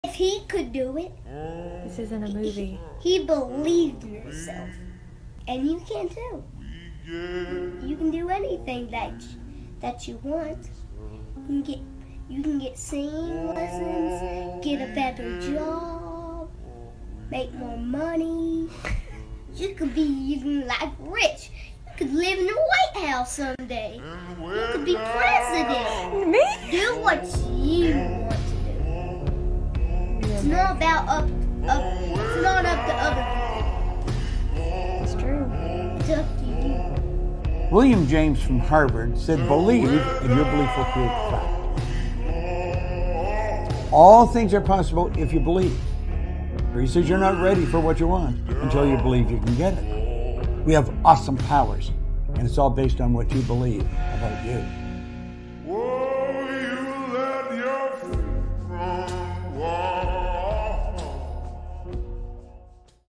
"If he can do it, you can too!" | YOUNG KID MOTIVATIONAL SPEECH
Welcome to a powerful journey of motivation and self-belief with "If he can do it, you can too!" - a YOUNG KID MOTIVATIONAL SPEECH that will inspire you to reach for the stars.

In this heartwarming video, a young visionary shares his unwavering belief that anything is achievable with hard work and persistent faith in oneself. This isn't just a speech; it's a call to action for everyone who dreams but hesitates, who wishes but waits.